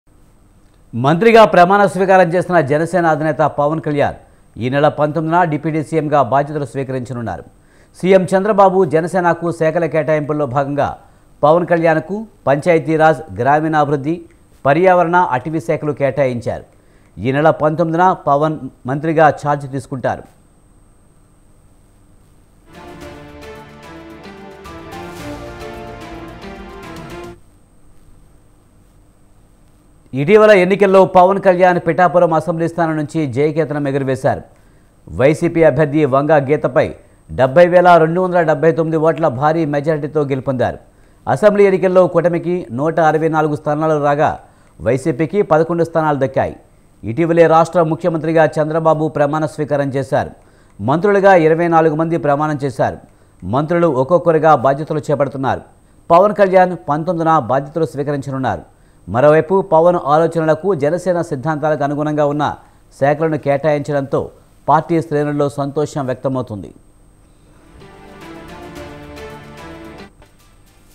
Pawan will take charge as Deputy CM on 19th of this month |swatantralive |pawankalyan |deputycm
Pawan will take charge as Deputy CM on 19th of this month